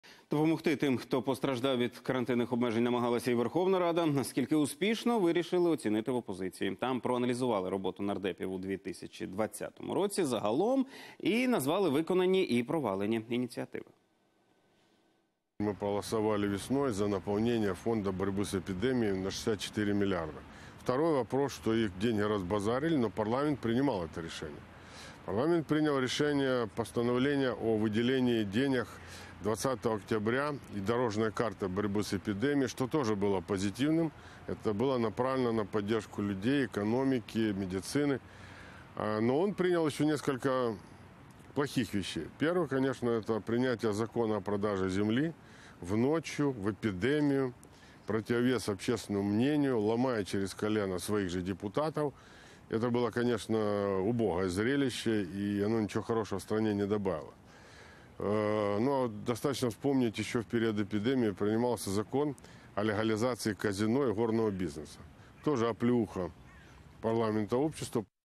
Парламентські підсумки: в ОПЗЖ оцінили роботу депутатів за 2020 рік - Юрій Бойко
Виконані і провалені ініціативи Верховної ради - які рішення підуть на користь українцям, а які навпаки?